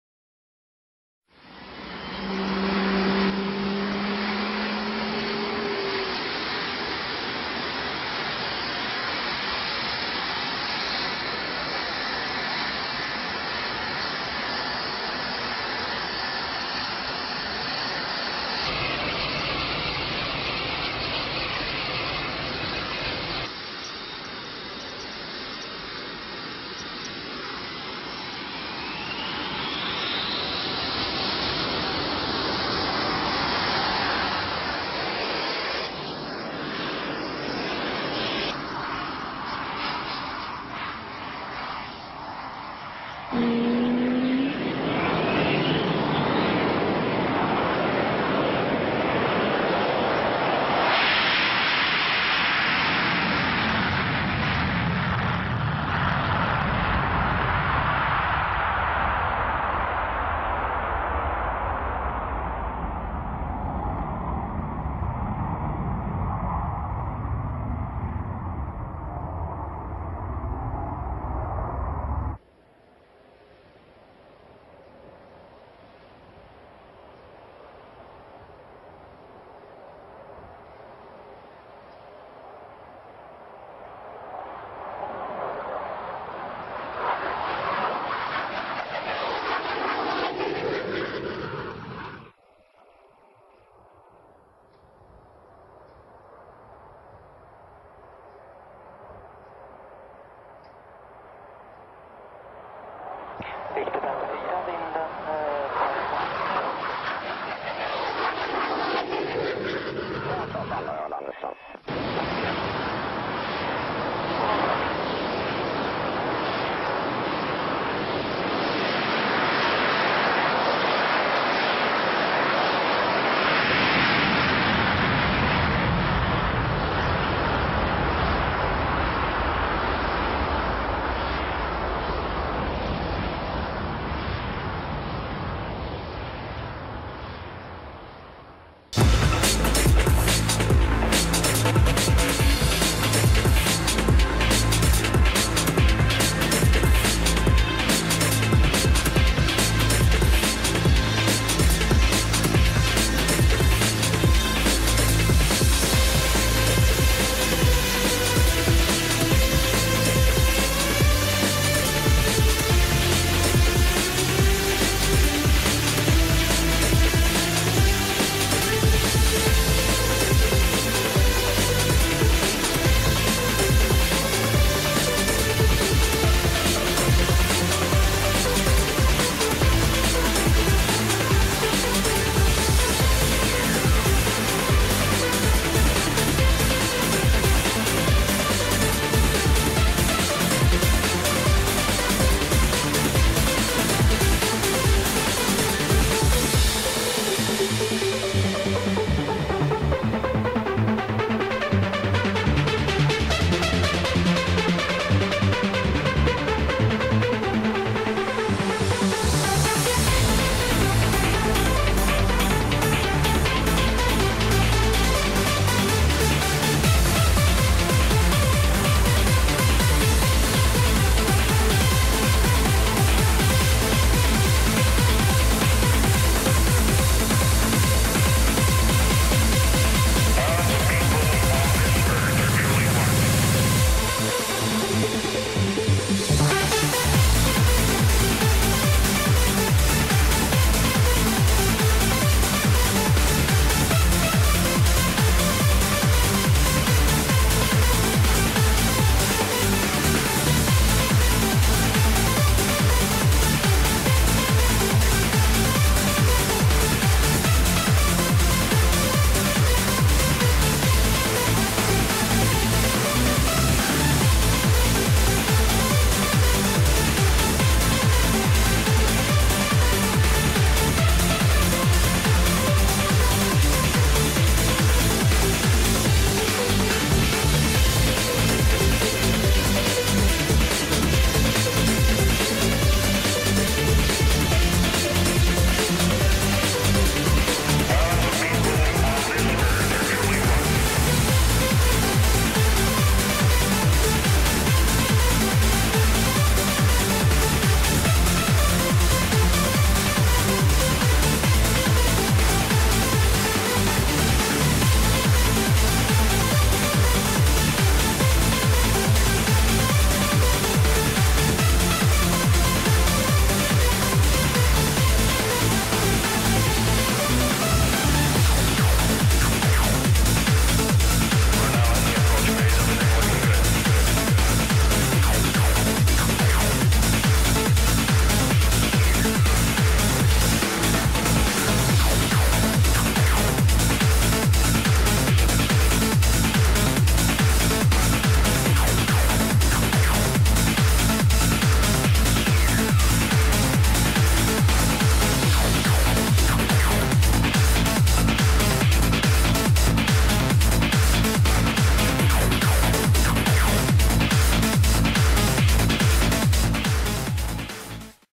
JAS 39 Gripen (1)
The Saab JAS 39 Gripen (English: Griffin) is a fighter aircraft manufactured by the Swedish aerospace company Saab. Gripen International acts as a prime contracting organisation and is responsible for marketing, selling and supporting the Gripen fighter around the world.

The aircraft is in service with the Swedish Air Force, the Czech Air Force, the Hungarian Air Force and the South African Air Force, and has been ordered by the Royal Thai Air Force. A total of 236 Gripens have been ordered as of 2008.